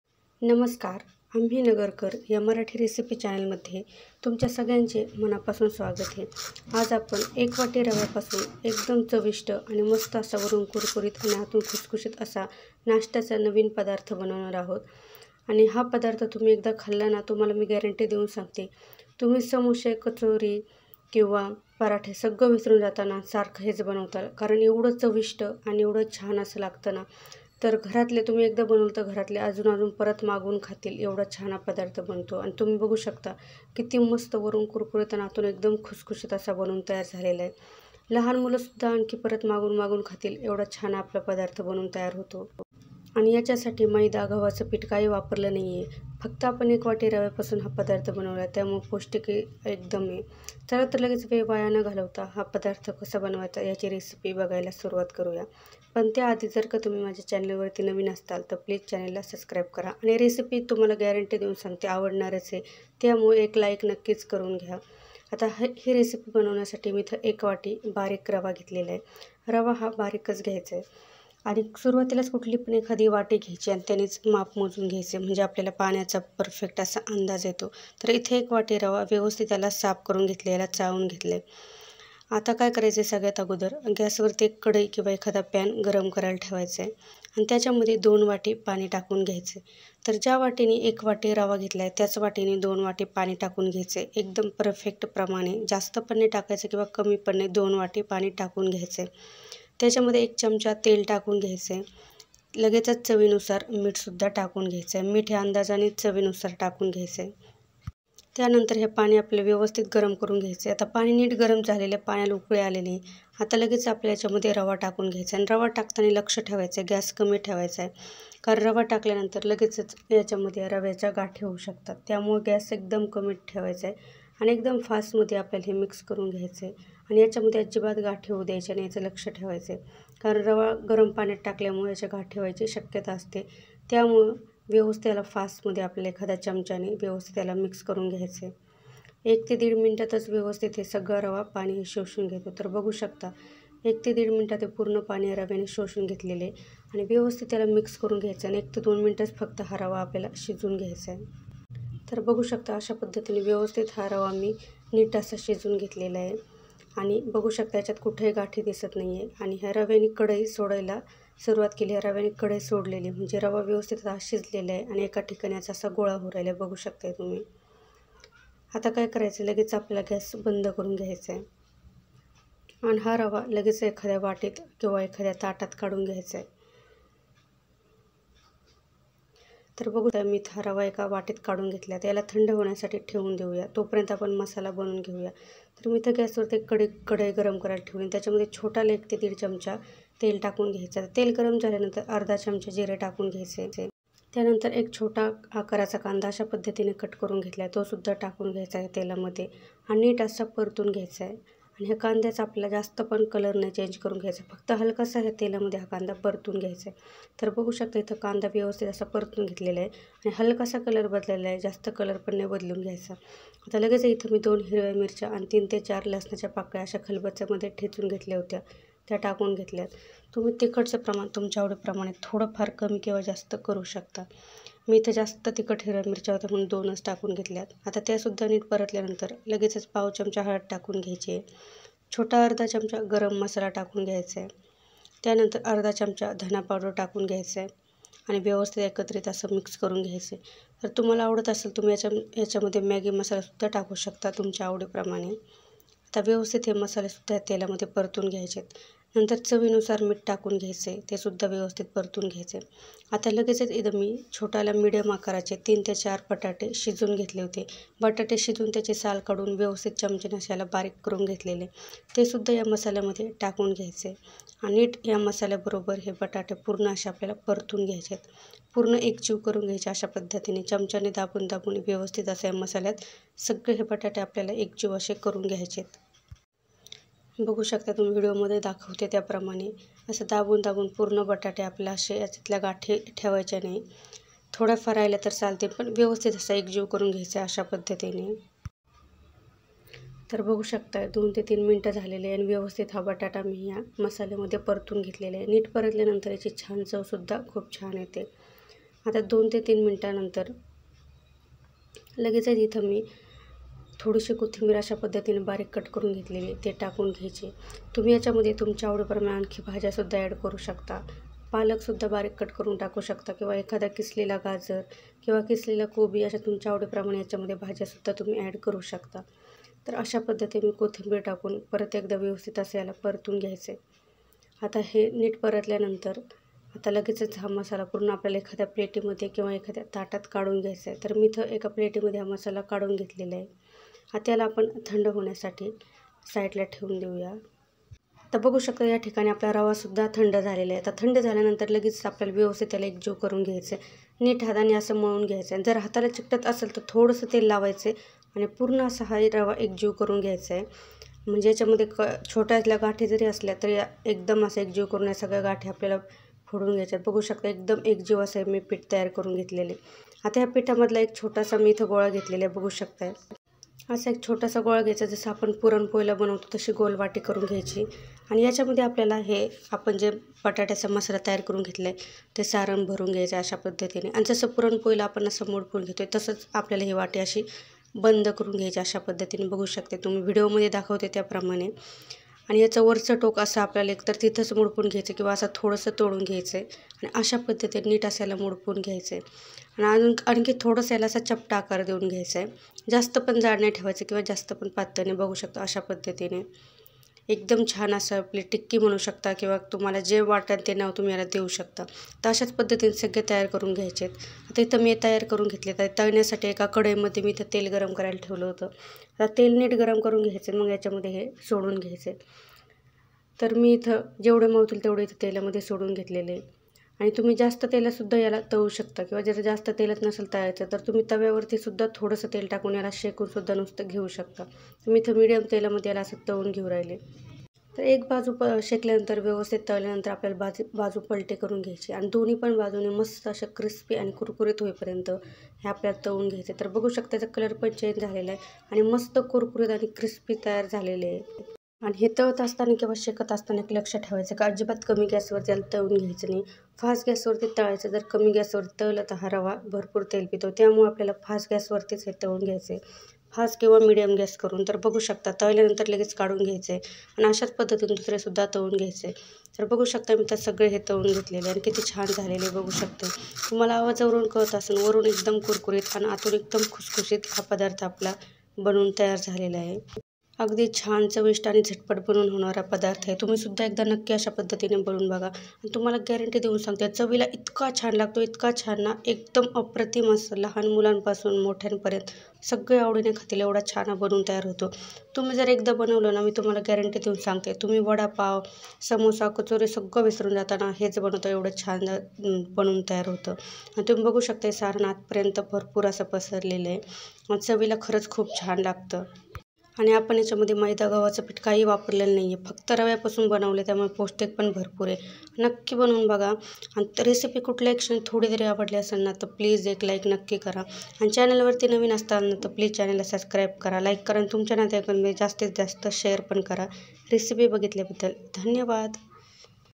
एक वाटी रव्यापासून कधीच खाल्ली नसेल एवढी भन्नाट आणि चविष्ट रेसिपीrava_aalu_newrecipe
गॅरंटी देऊन सांगते "समोसे, कचोरी सगळं विसरून जाताल एवढी चविष्ट "फक्त एक वाटी रव्यापासून"नवीन रेसिपी